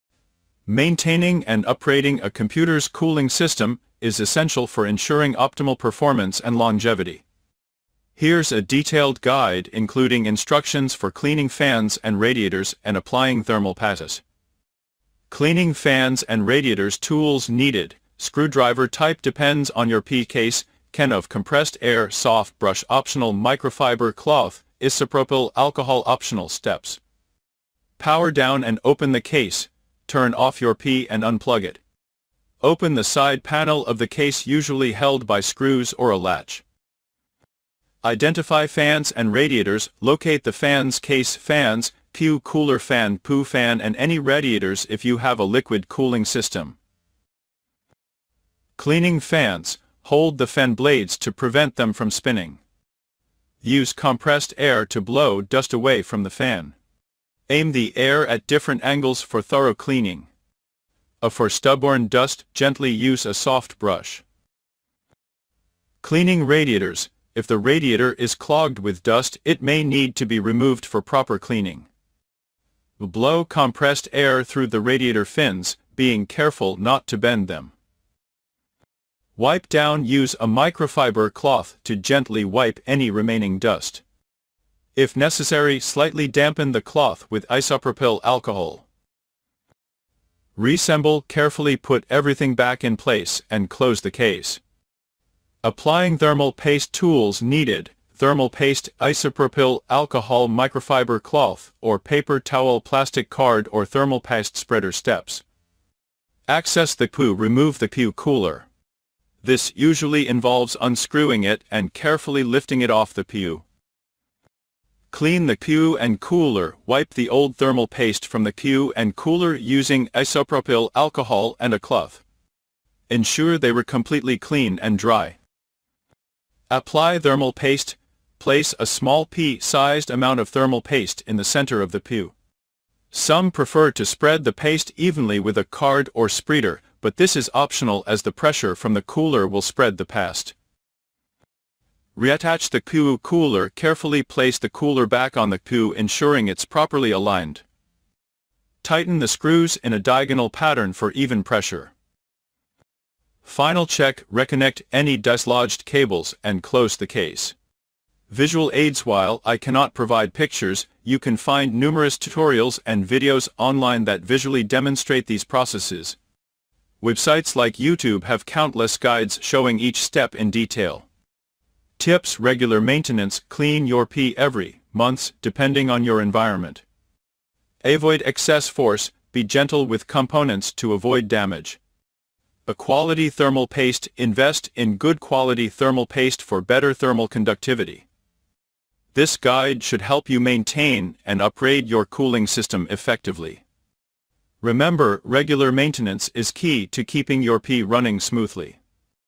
Как создать идеальную систему охлаждения для максимального производительности
For interesting content, see tourismtravelroad.blog
Dust Removal: Clean dust regularly to prevent overheating.
Thermal paste application: Improve heat dissipation from the CPU/GPU.
Battery Care: Optimize the charging process to extend battery life.
Software Update: Updates the operating system and applications.
Antivirus Protection: Install and update antivirus software to ensure security.
Hard disk health: Monitor and maintain hard disk performance.
Cooling system check: Ensure that fans and vents are functioning properly.
Physical cleaning: Regularly wipe the screen, keyboard, and external surfaces.
Data Backup: Backing up important files and documents on a regular basis.
Hardware upgrades: Consider upgrading the RAM or SSD to improve performance.